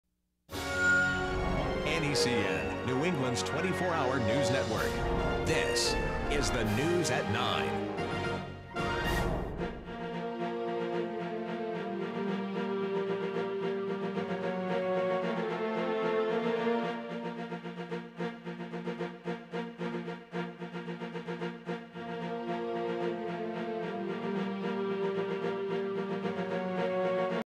News at 9
Testing out some new ideas for the News at 9 at NECN. Not RD Sahl and Beth Shelburne, but Ang and I standing in. Real anchors wear microphones, but somehow we didn't get the memo...